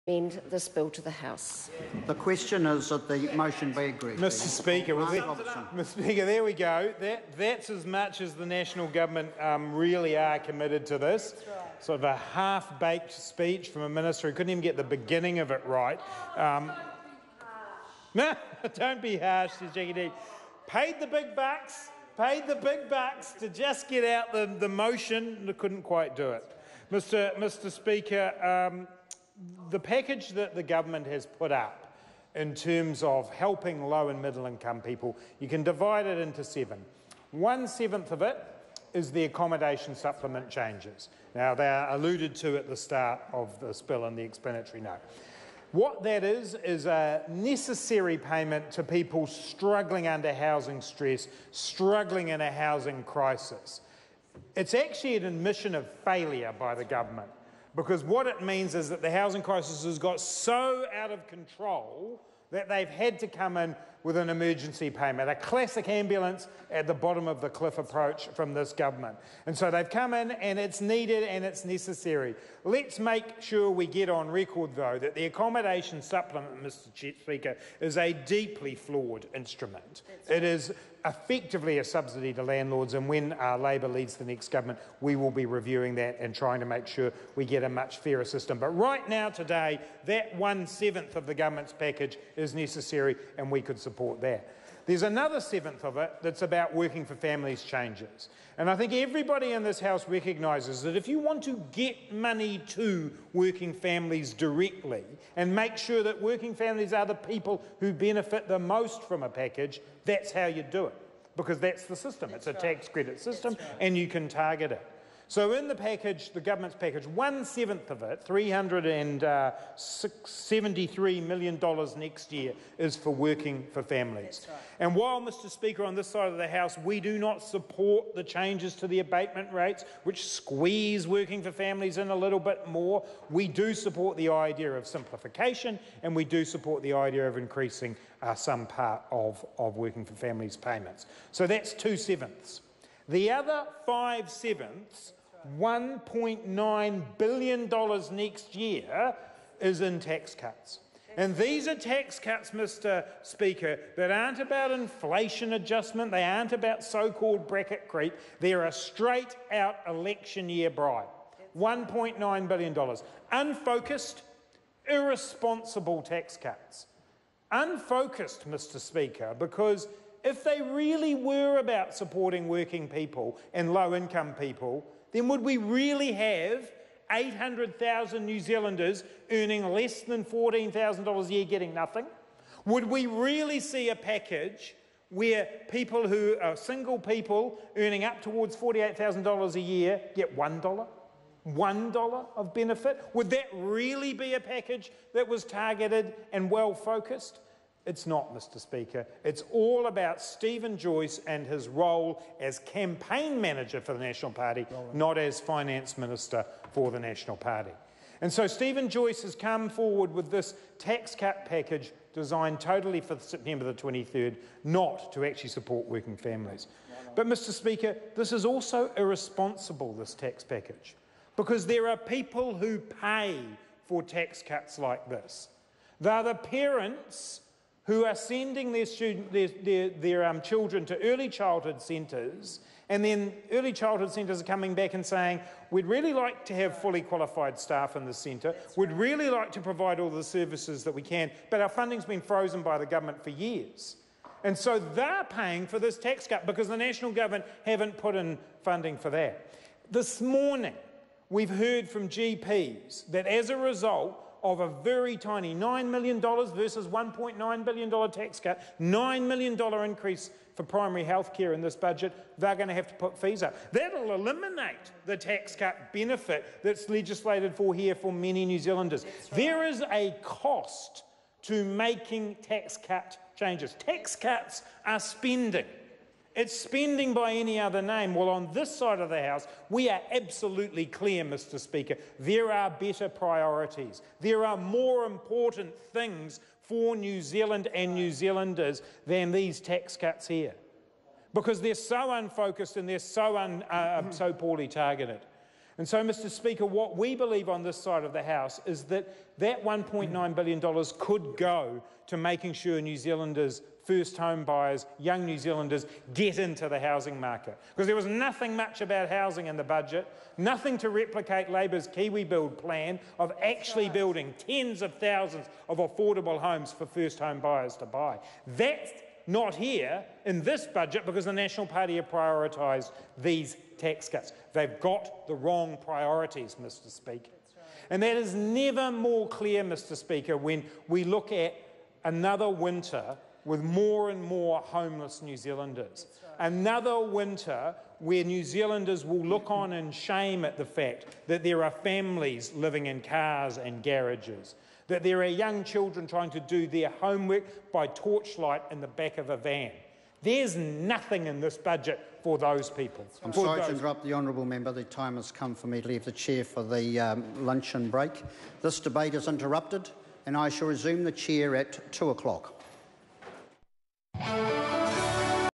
Taxation (Budget Measures: Family Incomes Package) Bill- Third Reading - Video 2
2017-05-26 - Taxation (Budget Measures: Family Incomes Package) Bill- Third Reading - Video 2

Grant Robertson,Lindsay Tisch